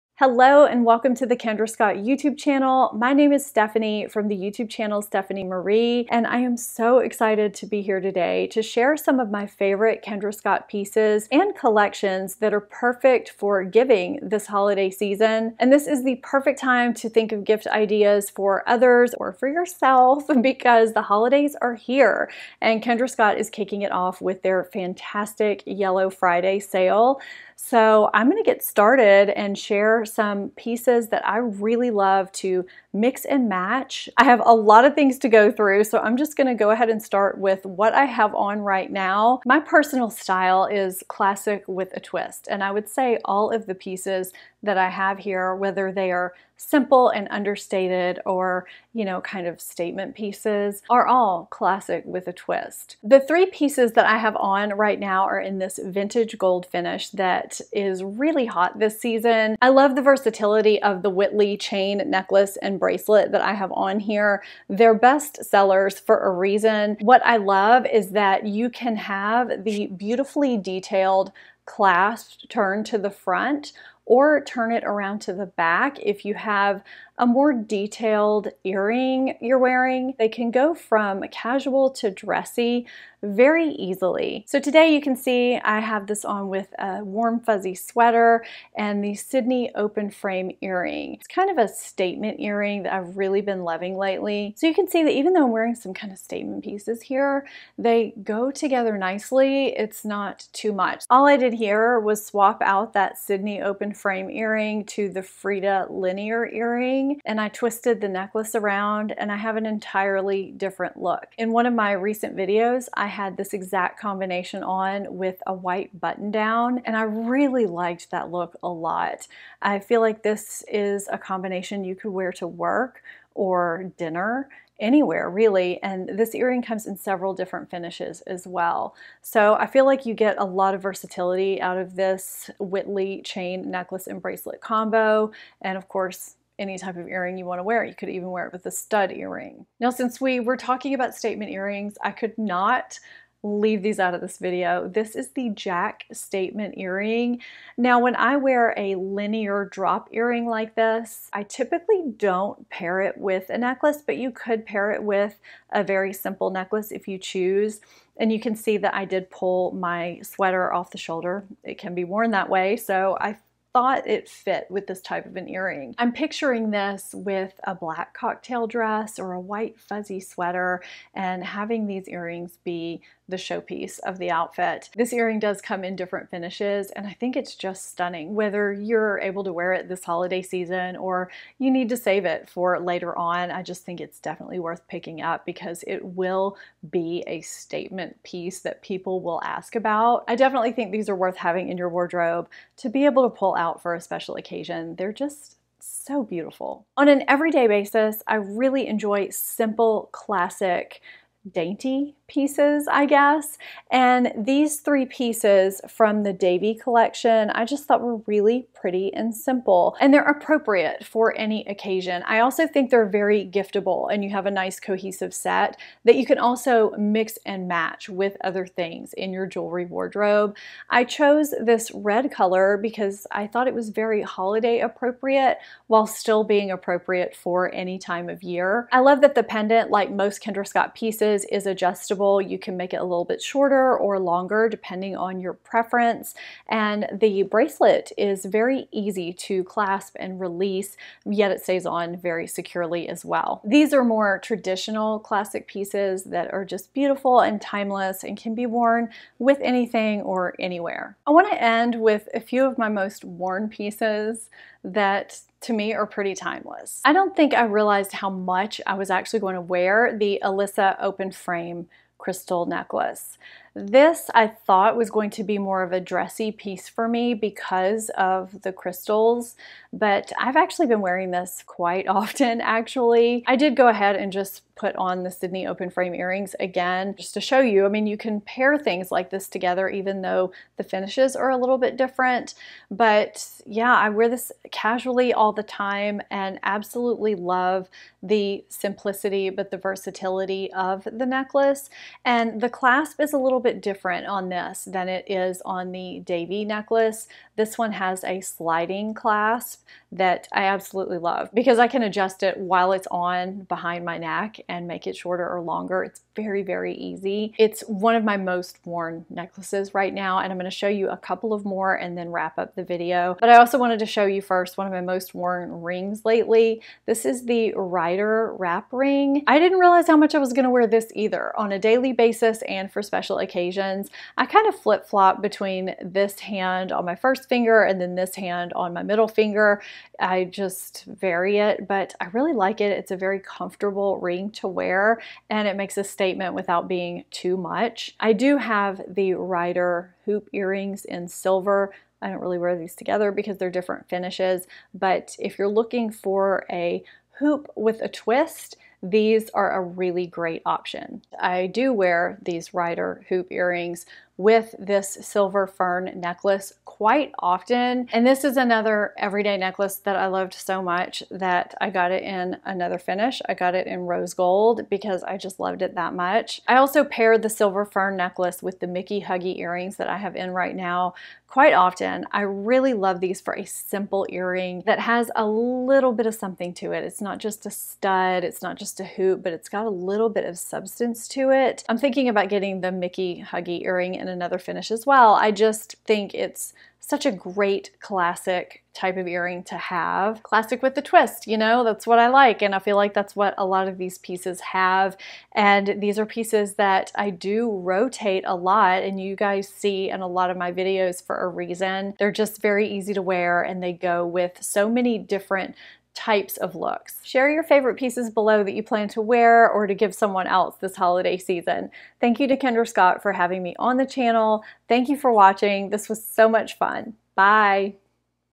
's Favorites from the New Kendra Scott Collection
Join @StephanieMarie as she walks you through all her new favorite styles! She shows you how easily you can style our pieces like the Whitley Chain Link necklace for day wear or at night. Looking for the perfect statement earrings to add festive flair to your Zoom Happy Hours? Stephanie will show you how to style any pair! Watch and enjoy.

Looking for tips on how to style jewelry, behind the scenes of jewelry, or to learn more about Kendra Scott?!